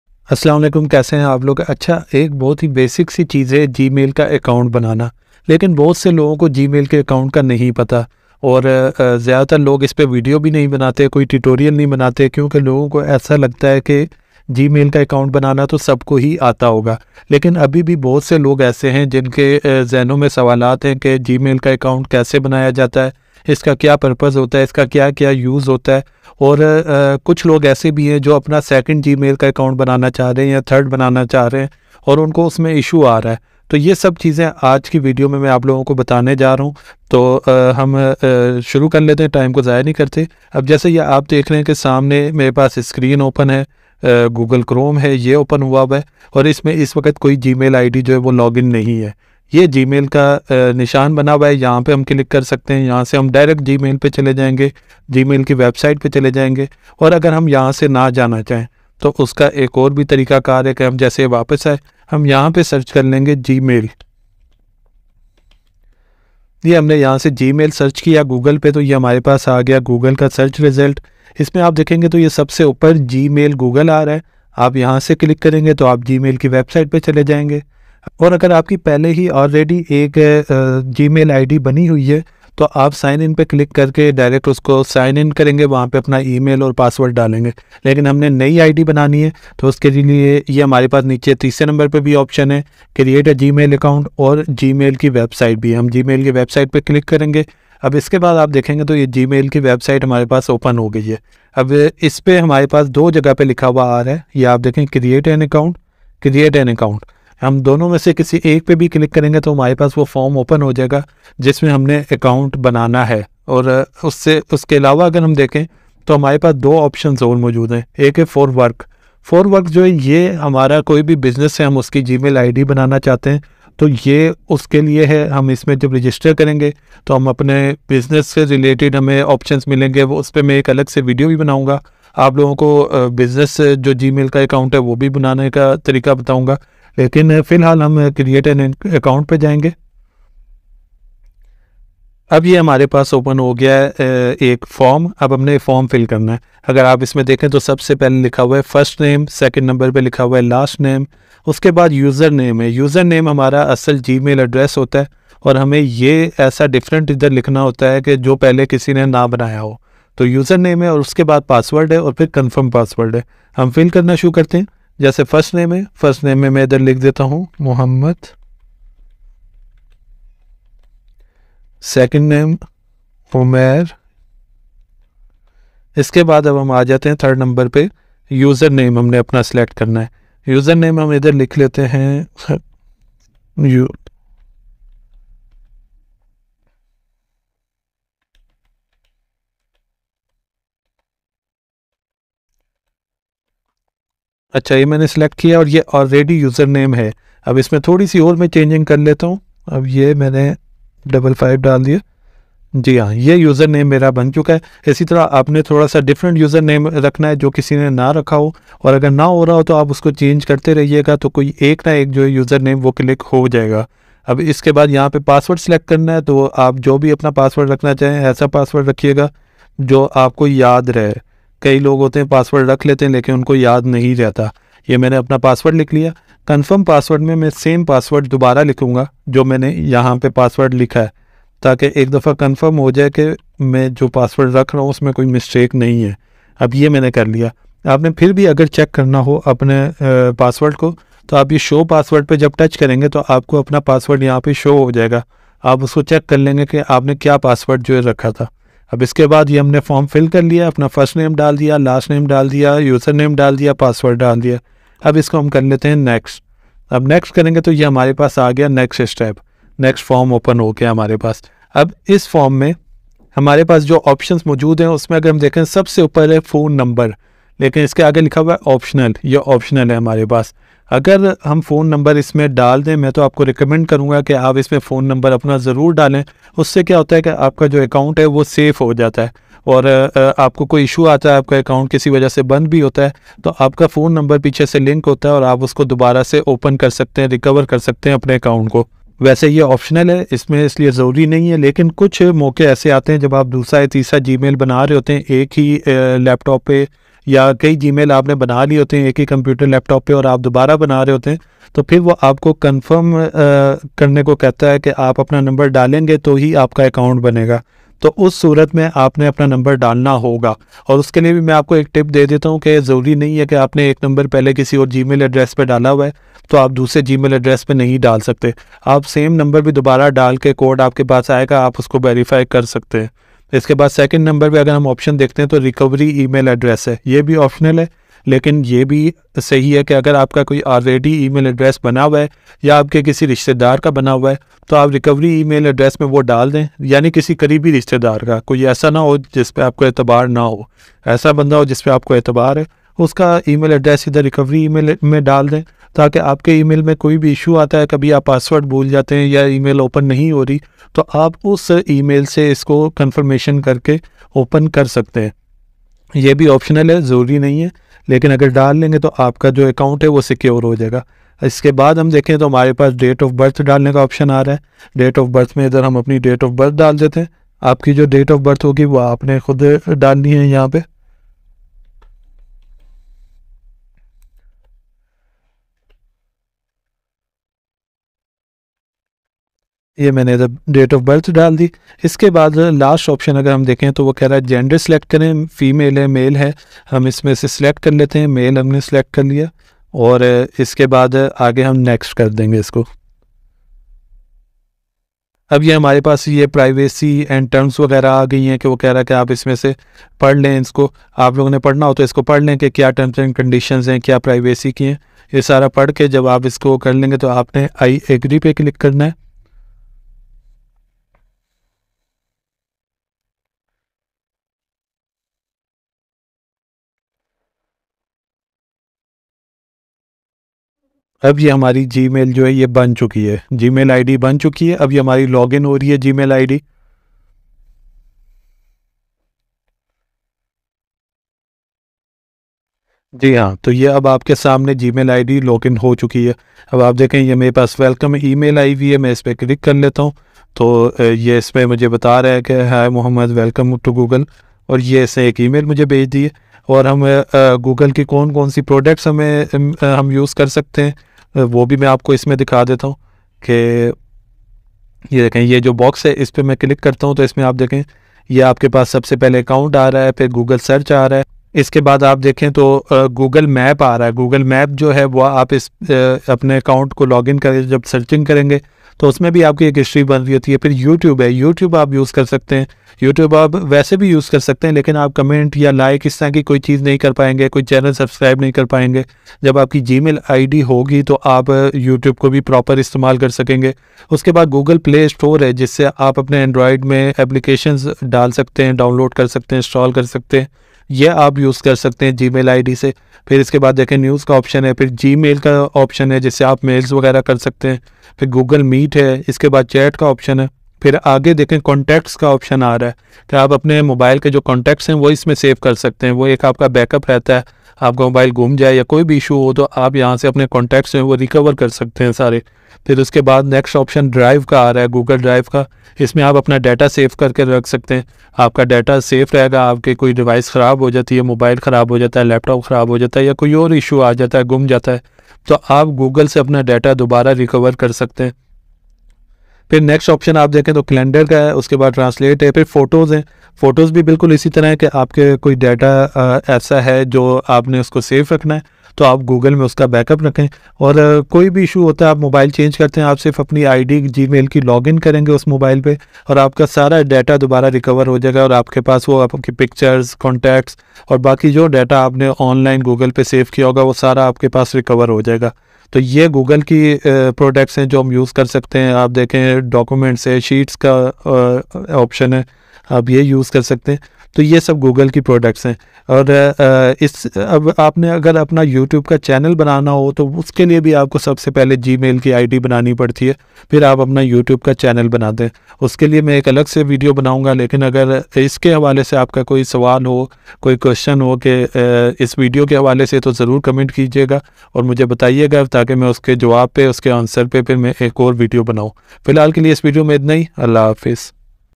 How to Create New Google Account | Create google account sign up
This video is all about creating a new Google account in this video you can learn how to open Google account and what is the use of Google new account